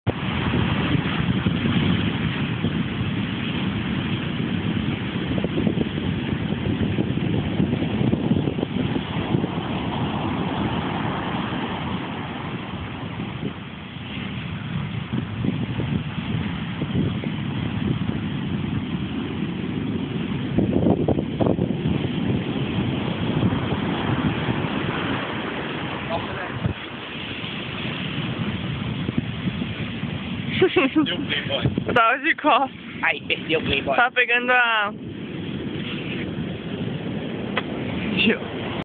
Paúba
Imagens de até onde o mar chegou, dia 25/09/2007
Filmagem Tarcila